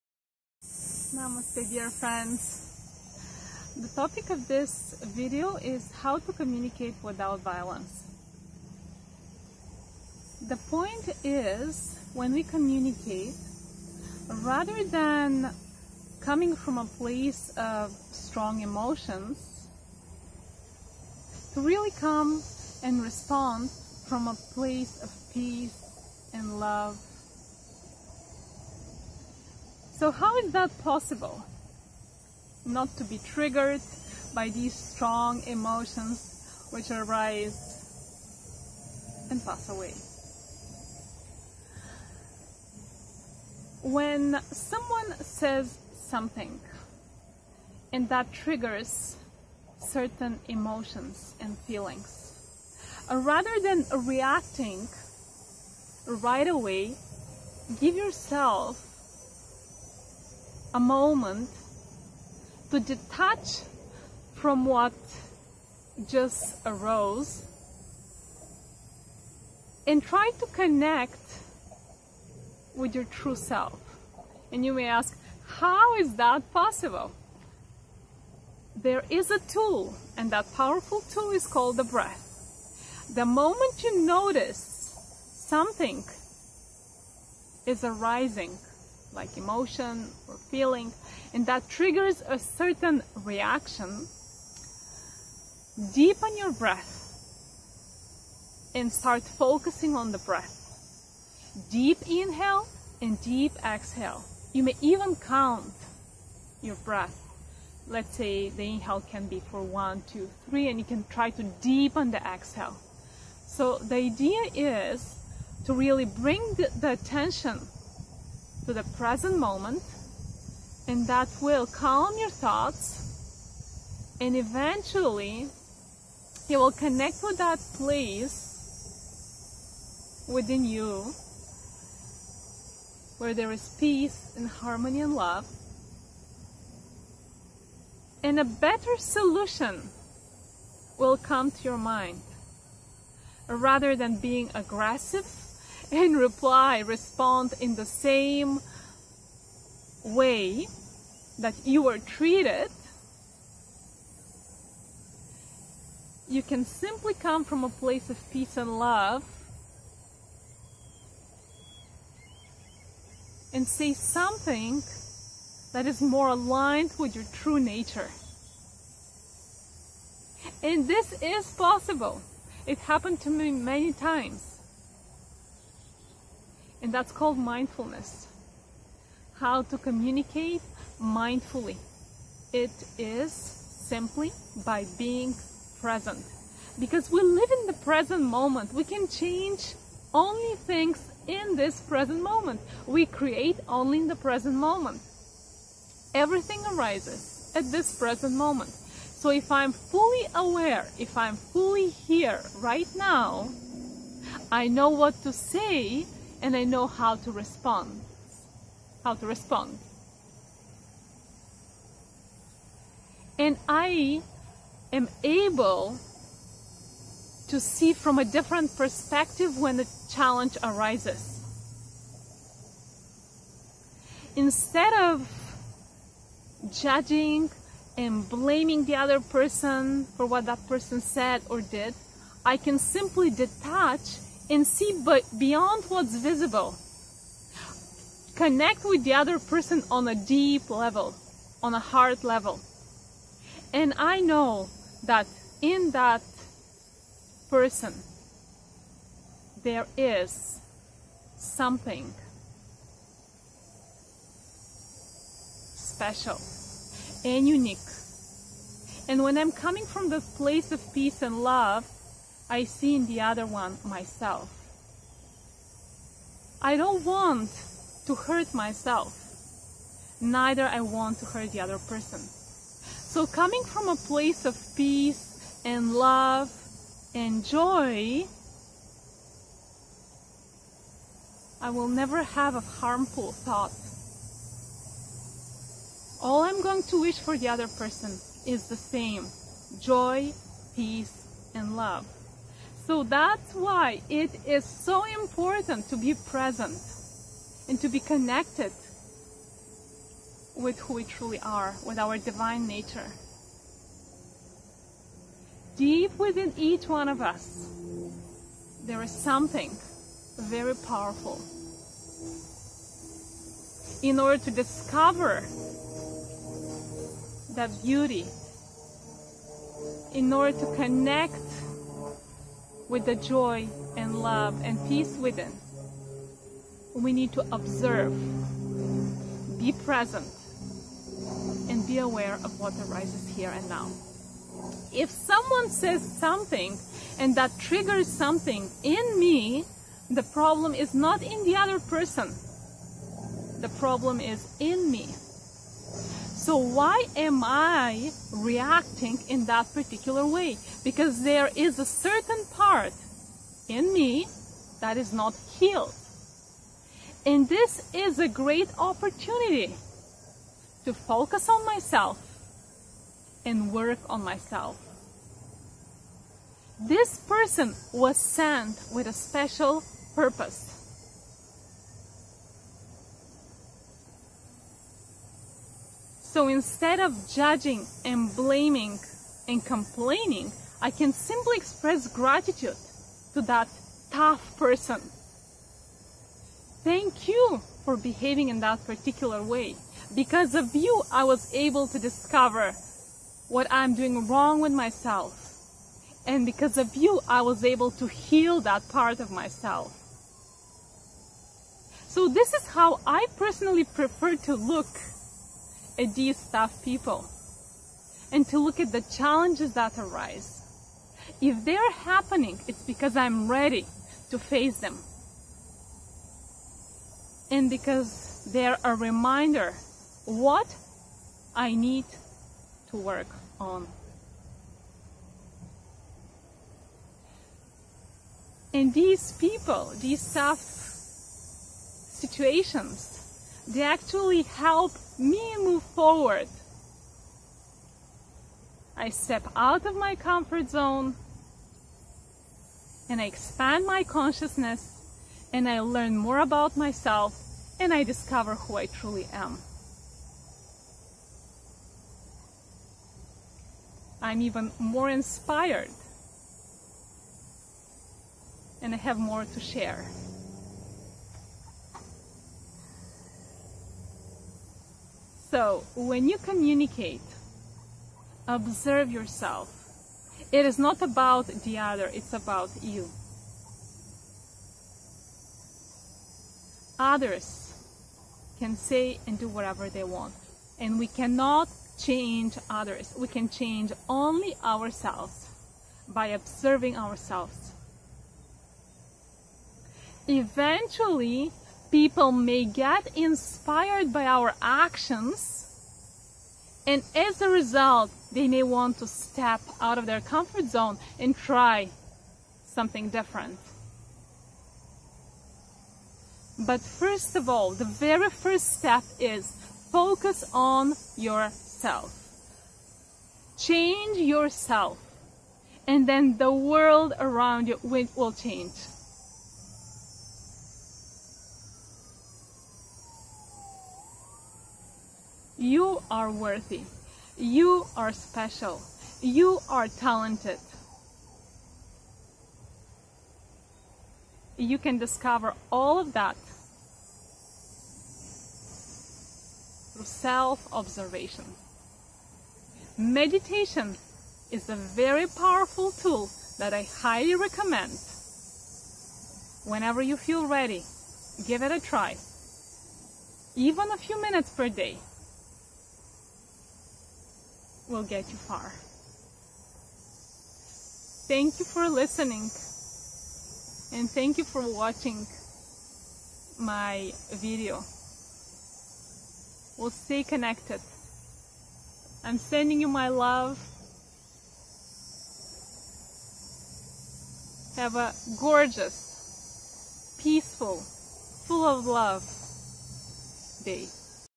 How to communicate without violence
Communicate with your heart and you will never harm anyone, including yourself. In my book, the Path, I talk about my journey and how I chose to make the shift. If interested in reading about my story, please check my